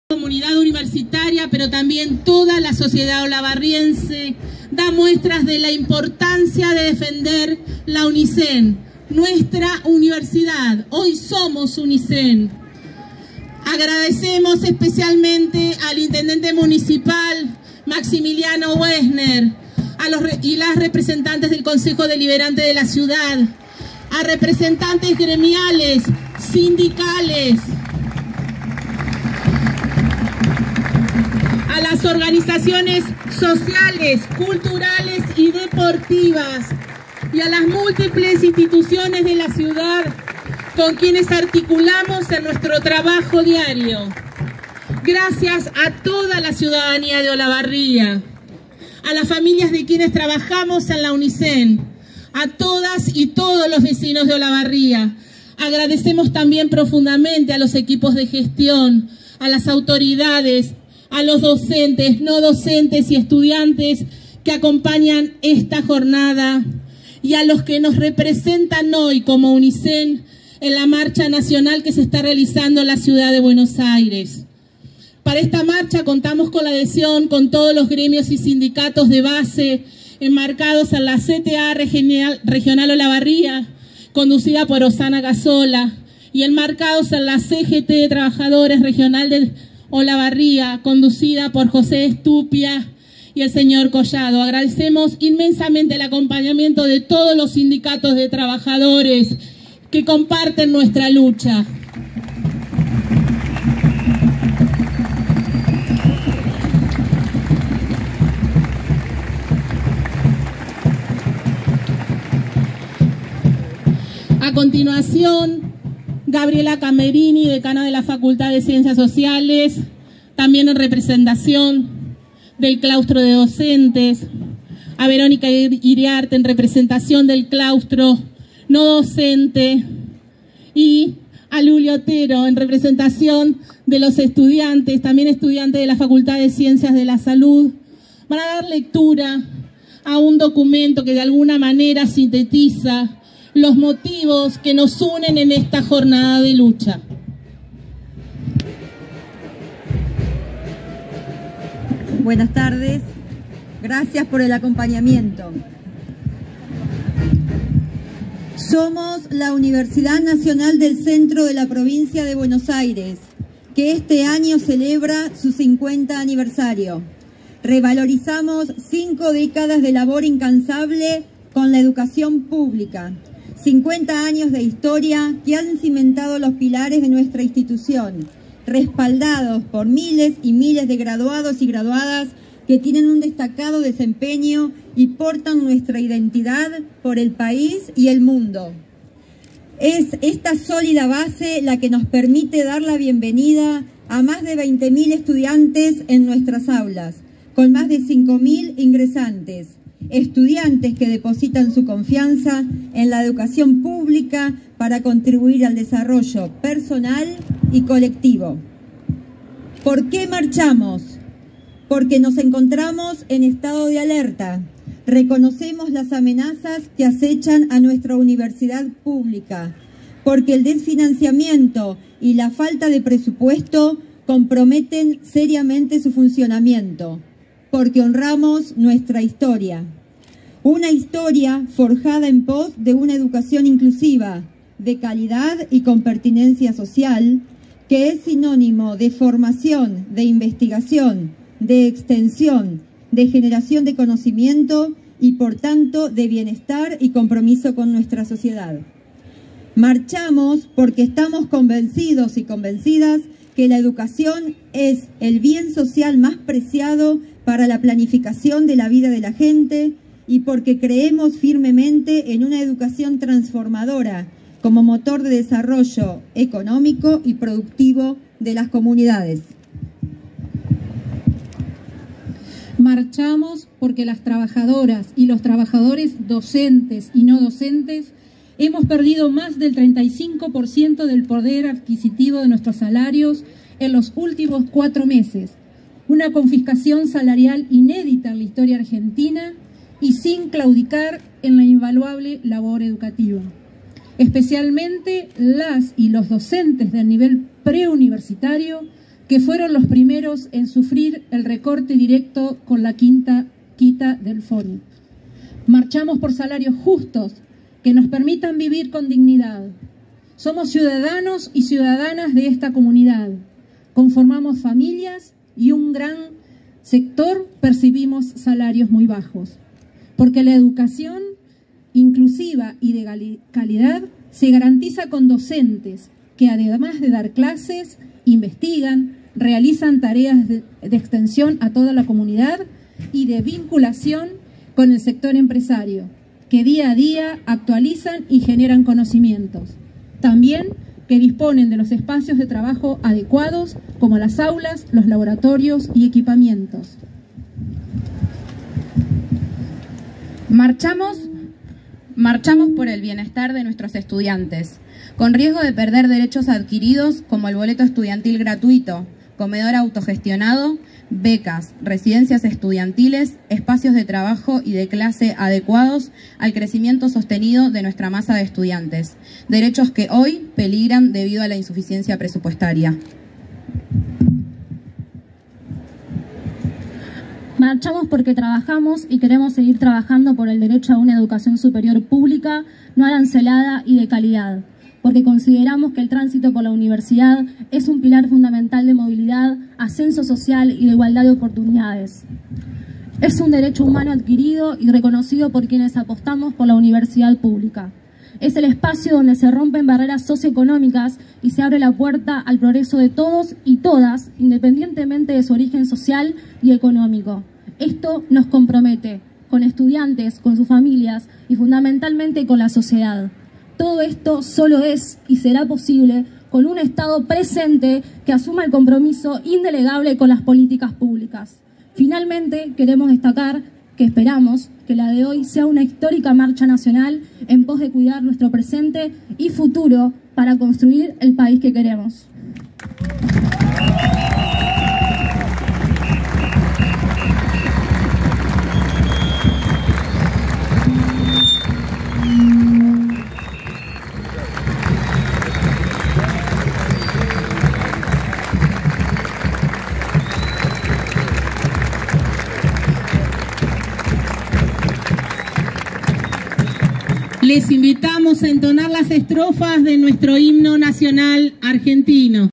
Lectura del petitorio durante la marcha en defensa de la Universidad Pública en Olavarría
En una jornada histórica de lucha y movilización del sistema universitario a nivel nacional, cientos de personas en Olavarría se congregaron en el Paseo Jesús Mendía para expresar su apoyo y solidaridad hacia la Universidad Pública y la UNICEN.

En el marco de la Marcha Federal Universitaria, referentes de distintos claustros de la UNCIEN, incluyendo no docentes, docentes, autoridades y estudiantes, se unieron para leer un documento que resume los motivos de la jornada y destaca el papel fundamental de la educación en la sociedad, señalando que contribuye al desarrollo tanto personal como colectivo.

El mensaje fue dirigido a diversas instancias y actores de la comunidad, incluyendo al Intendente Municipal y Representantes del Concejo Deliberante, Representantes gremiales, Organizaciones Sociales, fuerzas políticas, Instituciones Educativas, ciudadanía en general, familias de quienes trabajan en la UNICEN, autoridades, docentes, no docentes y estudiantes que se sumaron a la jornada, así como a aquellos que representan a la UNICEN en la ciudad de Buenos Aires.

La lectura de este documento resaltó la importancia de defender y abrazar a la Universidad Pública y a la UNICEN, reconociendo su rol esencial en la construcción de una sociedad más justa, igualitaria y democrática. La movilización reflejó el compromiso y la unidad de la comunidad universitaria y de toda la sociedad en la defensa de la educación pública como pilar fundamental para el desarrollo de nuestro país.